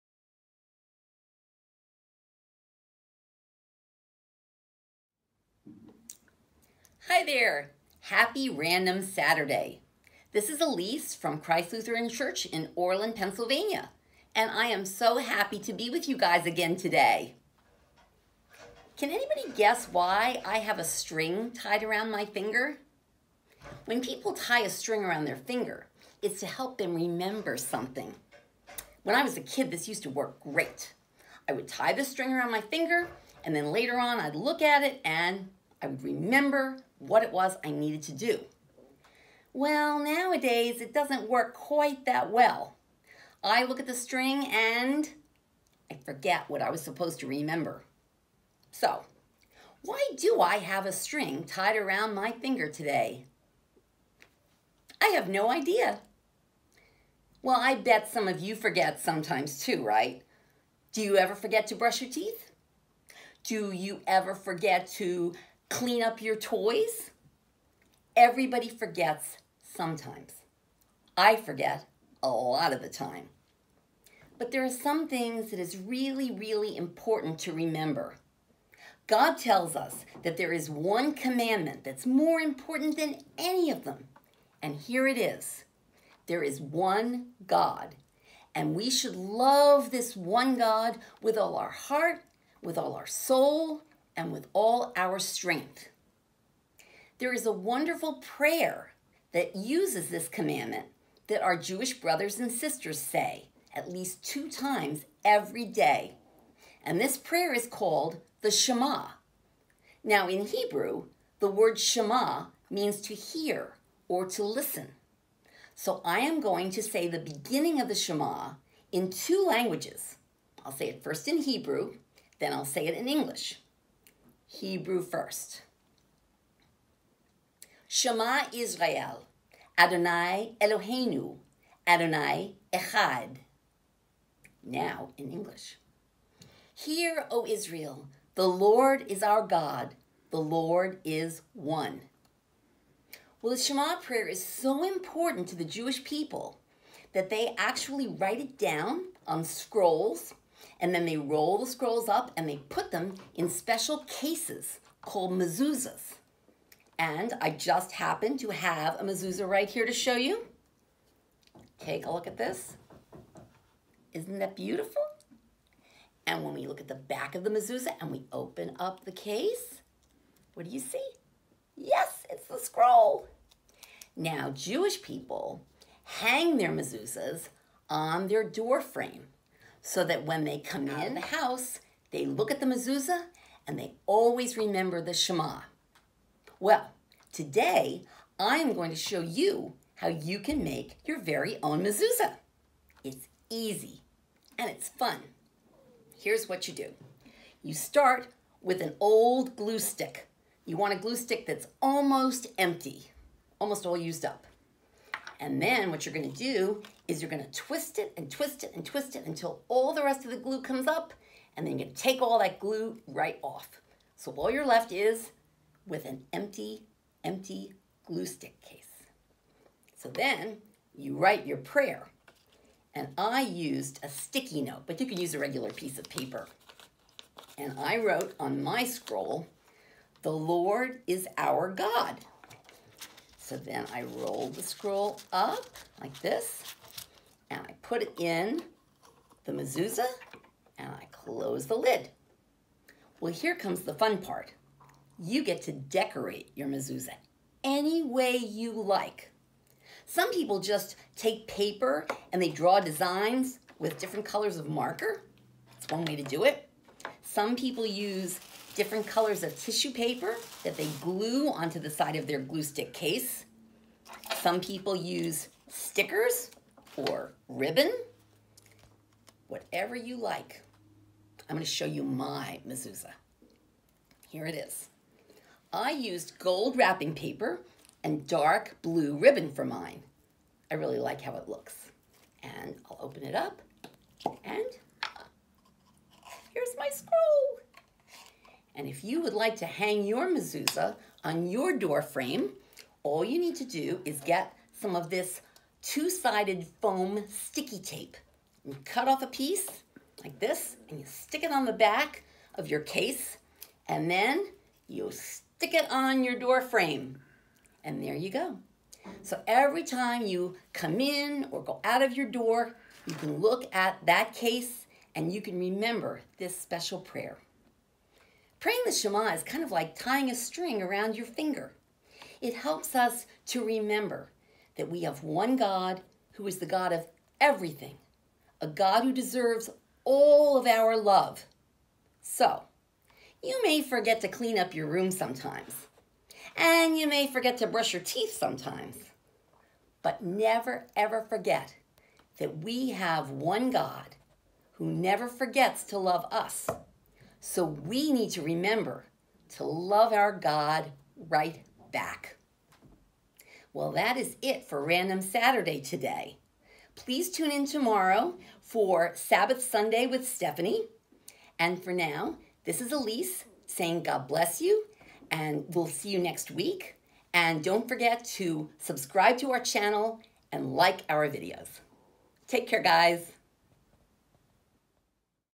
Faith Finders Random Saturday: The Shema
Let's learn about this very special prayer from the Bible! The Shema reminds us that we have one God, and we should love our God with all of ourselves. We'll also make our own mezuzahs!!